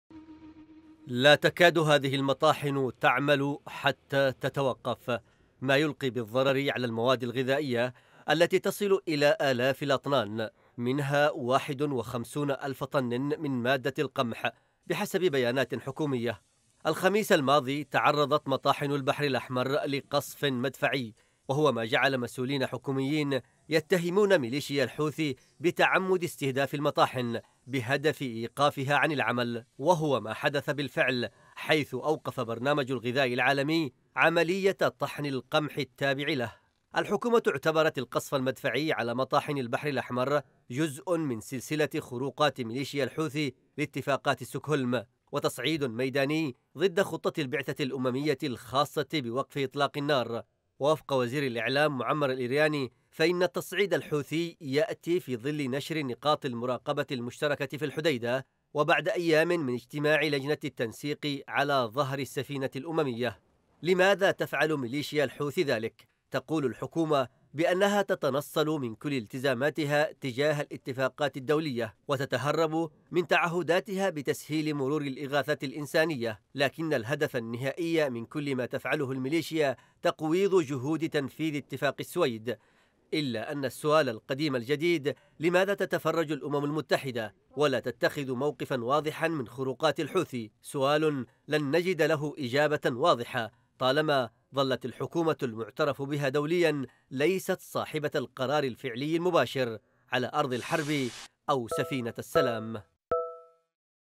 برنامج الأغذية العالمي يعلن وقف عملية طحن القمح بعد تعرض الصوامع لقصف الحوثيين | تقرير: محمد اللطيفي
استنكرت الحكومةُ استهداف مليشيا الحوثي لمطاحن البحر الأحمر في الحديدة، محملةً الأمم المتحدة مسؤولية وقف هذه الاعتداءات.
وقال وزير الإعلام معمر الإرياني في تصريح لوكالة سبأ الحكومية إن المطاحن تضم آلاف الأطنان من المواد الغذائية التابعة لبرنامج الأغذية العالمي، منها واحد وخمسون ألف طن من القمح تم تجهيزُها مؤخرا للتوزيع على المحتاجين.
ودعا الإرياني المبعوثَ الأممي ورئيسَ لجنة إعادة الانتشار وفريق الرقابة الأممي لتحمل مسئولياتهم واتخاذ موقف واضح من هذه الاعتداءات...